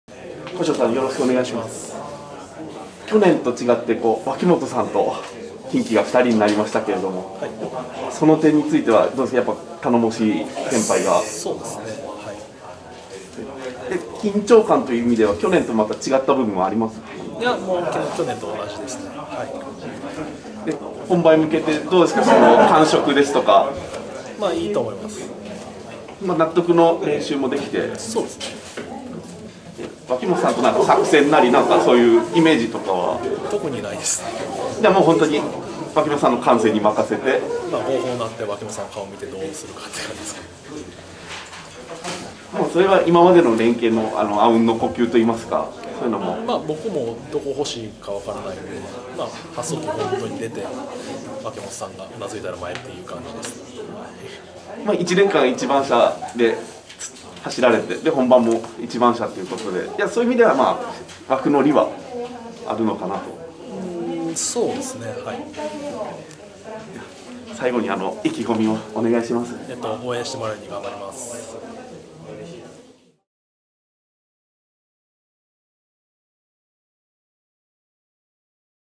【KEIRINグランプリ】 古性優作 号砲が鳴って脇本さんの顔を見てどうするか
昨年は単騎まくりで優勝をもぎ取った古性優作だが、今年は脇本雄太の番手回り。阿吽の呼吸で好連係を見せる！